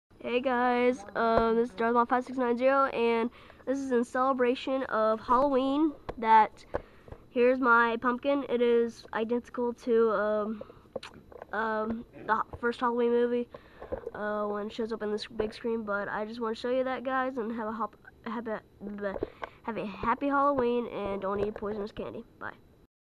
HALLOWEEN Movie Style Carved Pumpkin
Showing the pumpkin I carved to give honor to HALLOWEEN the Movie pumpkin. Like or Dislike, but please no rude comments.